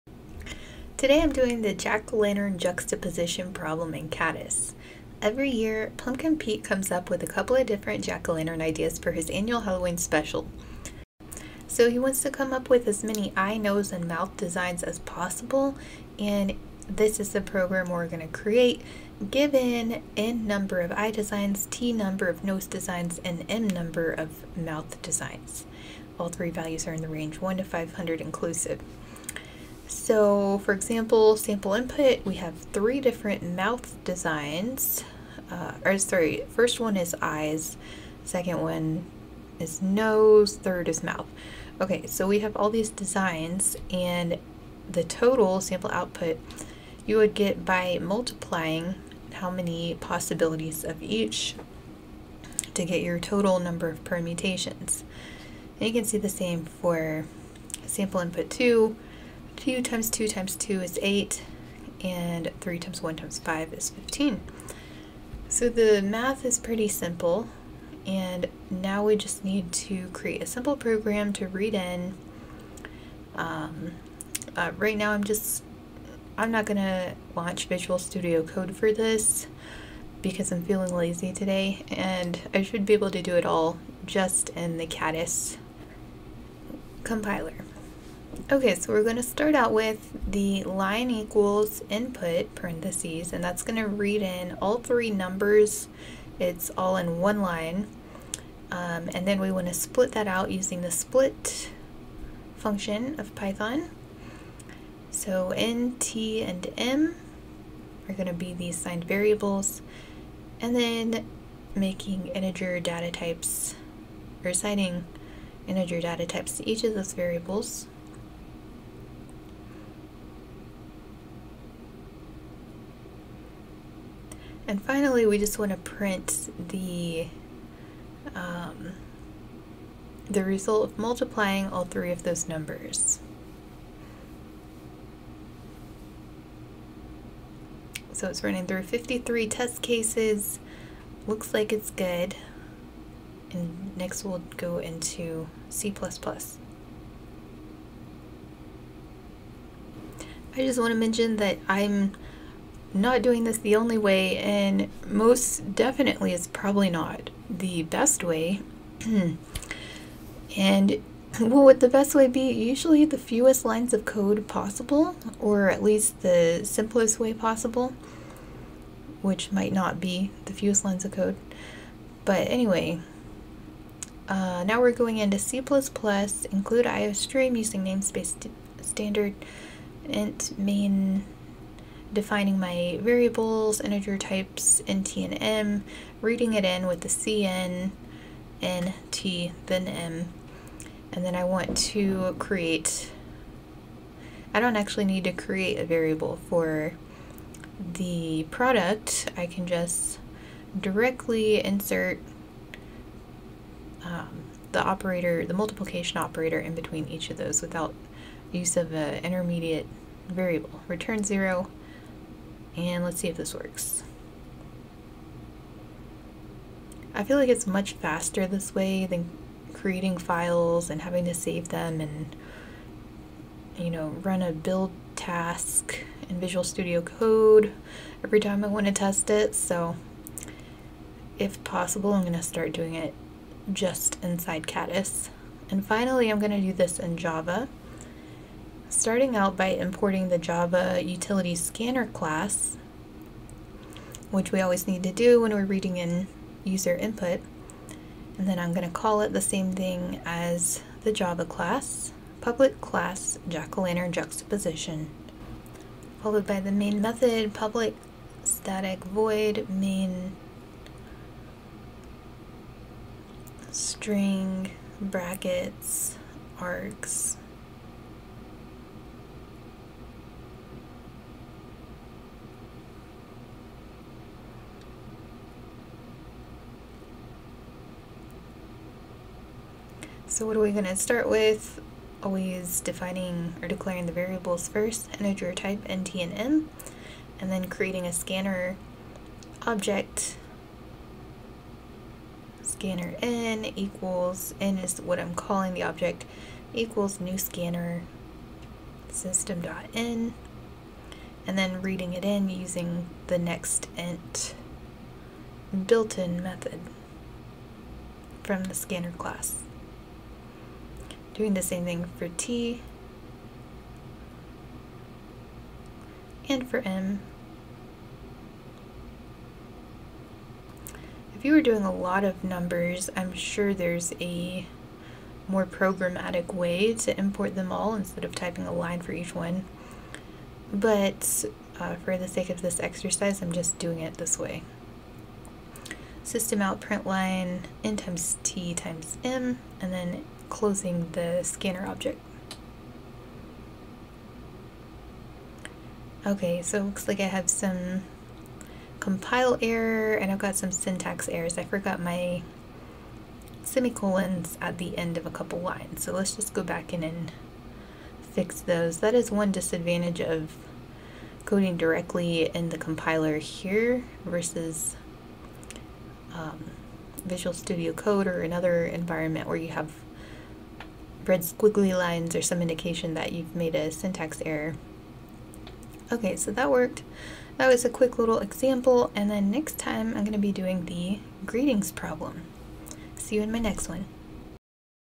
Learn to Code with Me: Kattis "Jack-o-lantern" Problem in Python, C++, and Java
Learn to code in the main 3 languages with me! Python 3, C++, and Java.

This is not meant to be an in depth coding lesson. It's more of a "practice along with me" to get more hands on experience. I assume some foundational knowledge of programming, but if not there are plenty of excellent and FREE coding instructors on YouTube to check out.